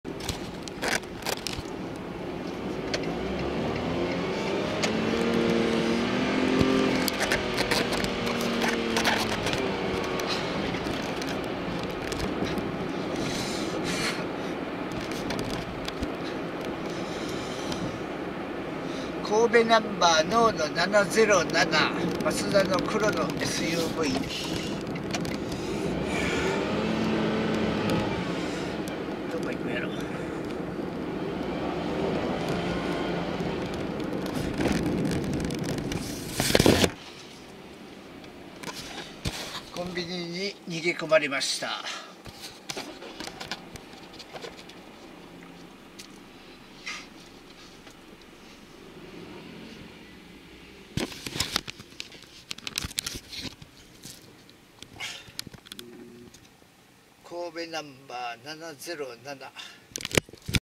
マツダのSUVが追いかけてきたので後ろに回って追いかけたらコンビニの中に逃げ込みました。黒い服を着た若い主婦みたいな人が乗っていました。
小さい子供がいるような若い主婦みたいな人もたくさんマスクで顔を隠して
マツダの車やゾロ目のナンバーを見せに来ます。こんな人に子供の教育ができるんですかね。